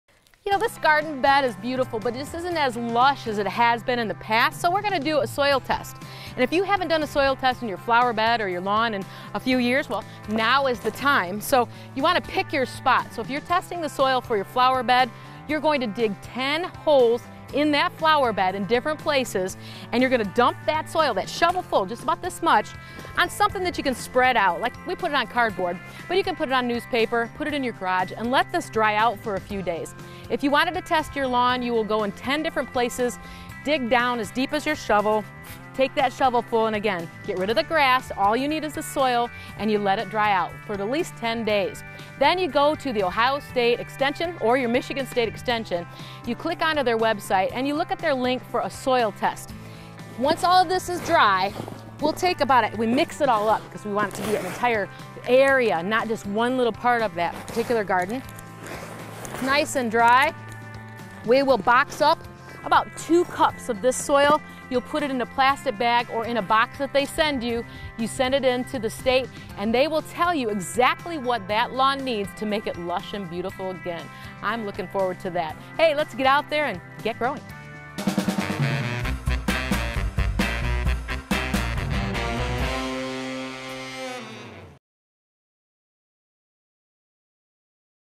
How to do a soil test | GET GROWING!
Get your soil tested. It's not hard to do. The farmers do it all the time, and so should you! Clean your trowel and bucket, then go around the yard and dig 10 small holes. They should be eight to ten inches deep. Dig out about half a cup of soil from each hole and put it in your clean bucket Make sure the holes are scattered around the turf part of your yard, especially in the weedy areas. Let the soil dry on newspapers for a couple days, then call your county extension office for instructions on where to send your soil sample. They will send information on what to put on your lawn to make the soil healthier.